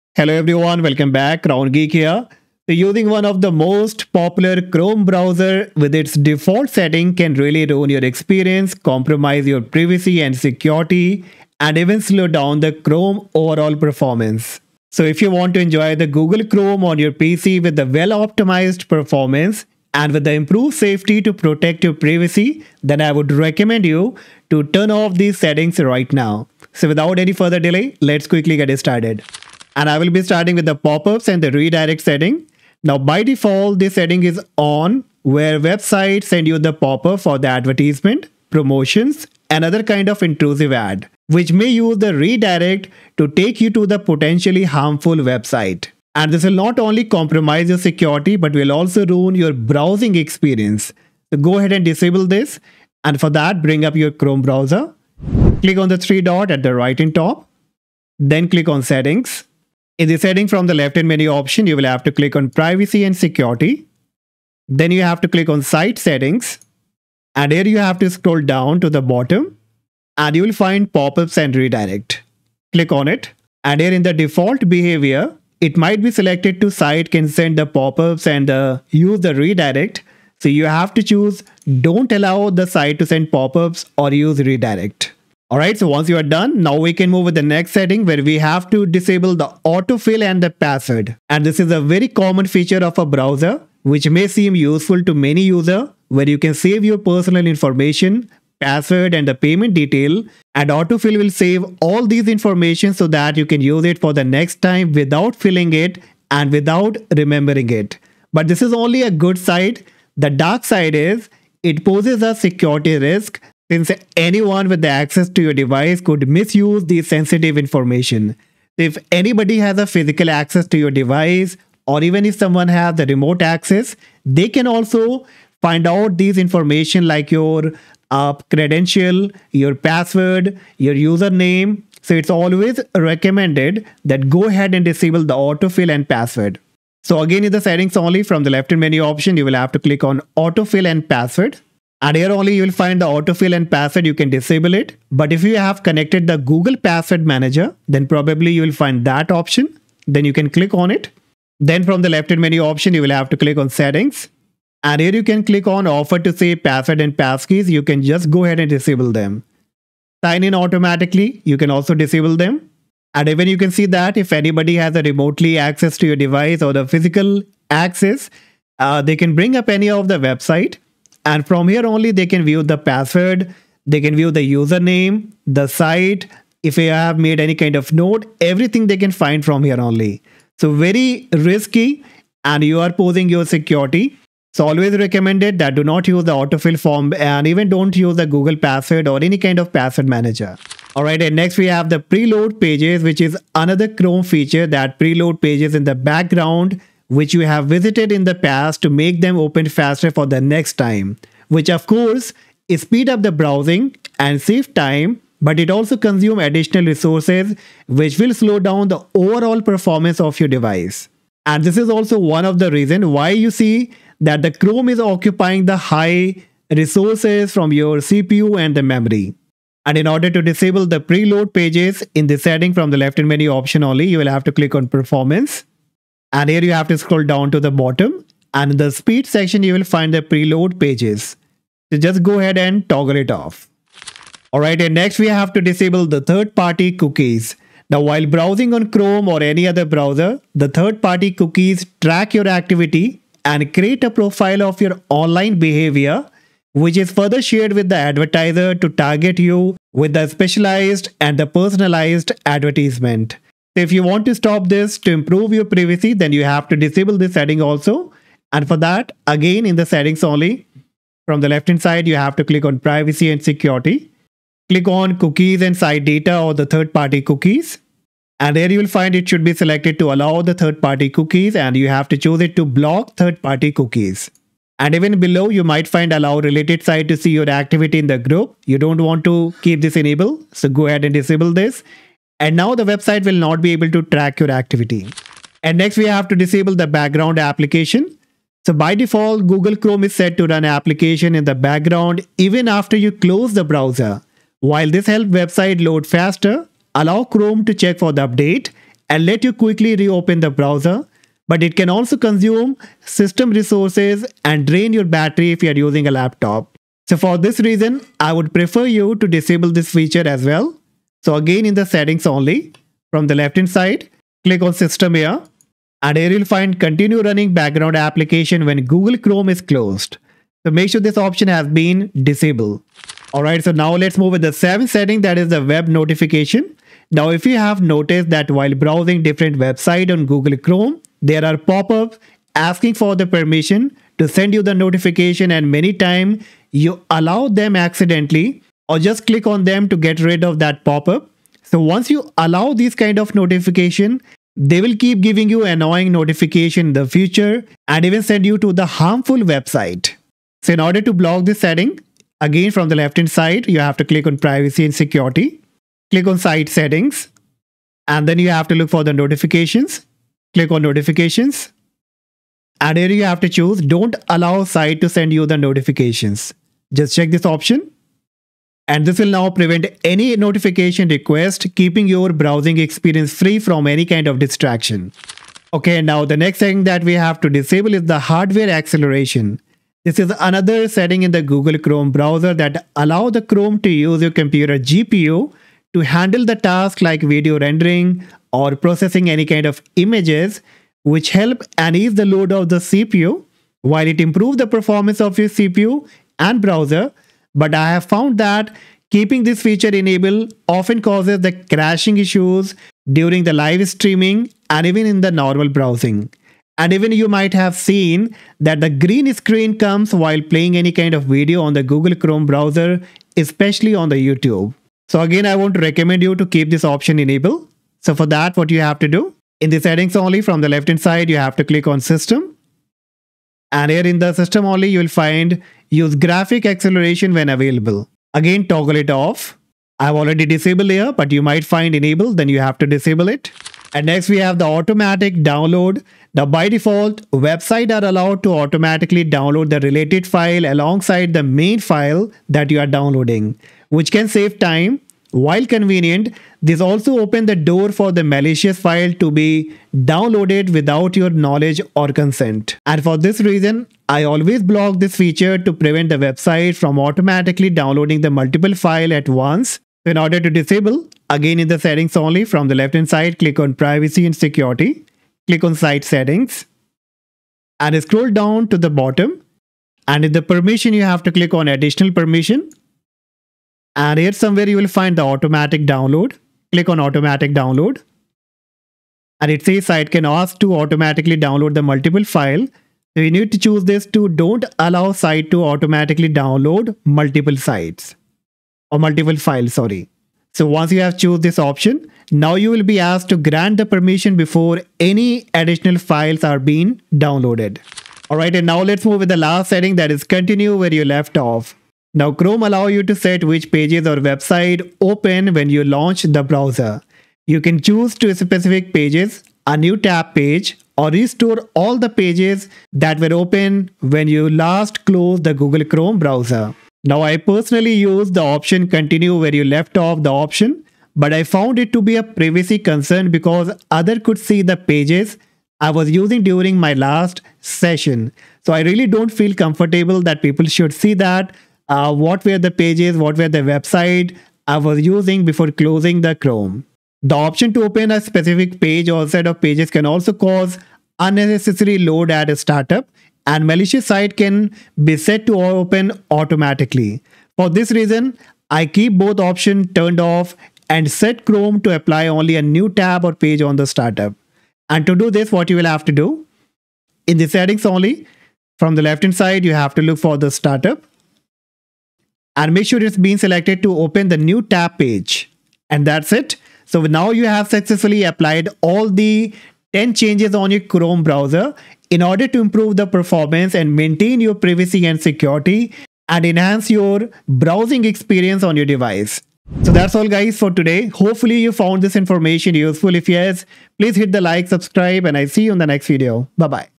10 Google CHROME Settings Always Keep DISABLED on your PC
10 Google CHROME Settings Always Keep DISABLED on your PC
So using one of the popular Chrome browsers with its default settings can Ruin your experience, compromise your Privacy and security, and
even slow down chrome performance.
So if you want to enjoy Google Chrome on your PC with well-optimized performance and improved safety to protect your privacy, turn off these 10 settings now.

▬▬▬▬▬▬▬▬▬ஜ۩۞۩ஜ▬▬▬▬▬▬▬▬▬
▬▬▬▬▬▬▬▬▬ஜ۩۞۩ஜ▬▬▬▬▬▬▬▬▬

☑ Watched the video!
☐ Liked?